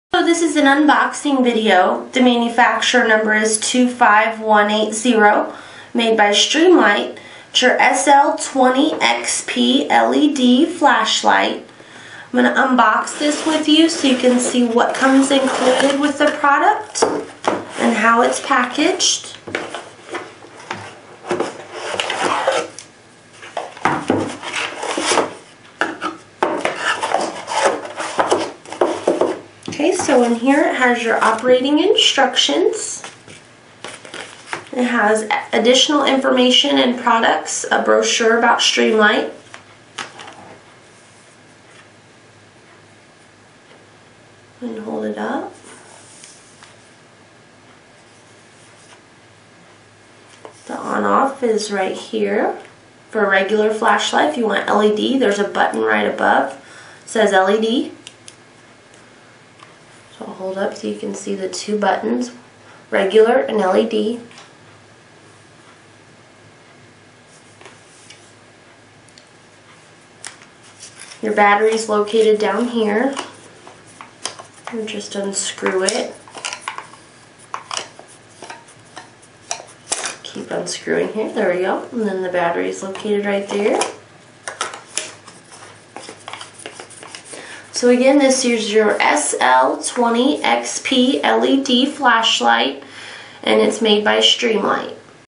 Streamlight SL20XP-LED 25180 SL20XP LED (With Out Charger) Yellow Shooting Gaming Unboxing
This is our unboxing video for the Streamlight SL20XP-LED SL20XP LED (With Out Charger) Yellow mfg#25180. At Wholesale Hunter